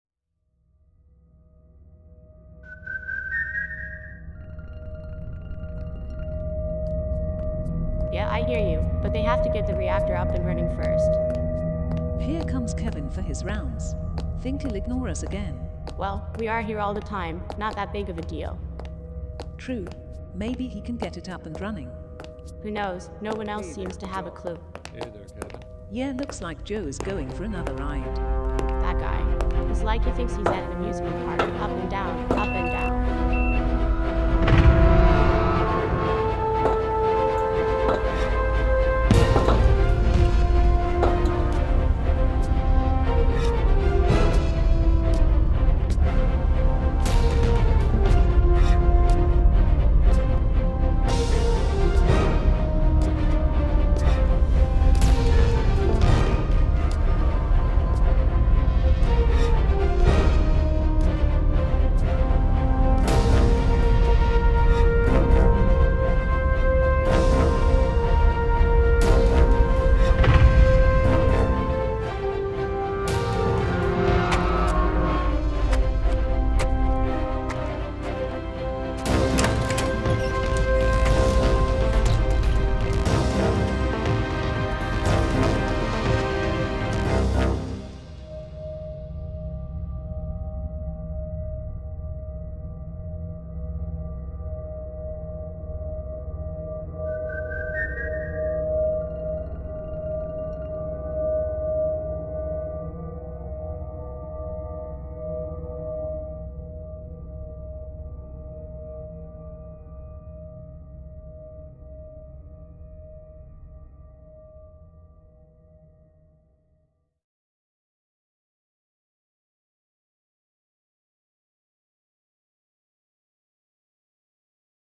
Going Up (take two) - Blender CGI (Eevee render) and Davinci Resolve green screen practice test...
Any questions, just ask. Link to the 1st version which has alittle "behind the scenes" of the green screen and modeling
Sound "design" and music by: SidewayUp / It's a Secret Music - Female voices: IBM Watson TTS (Text To Speech)

Rendered in Eevvee at 256 sample rate, 1835 frames, each about 30 secs to render for a total time of roughly 15 1/2 hours. Windows 10 machine, i9-9900K cpu, ZOTAC GAMING GeForce RTX 2070 and 64GB ram

For anyone interested, heres the video files. Raw video from camera (4k) and tif images (720p, already green screen keyed, ready for importing to Blender).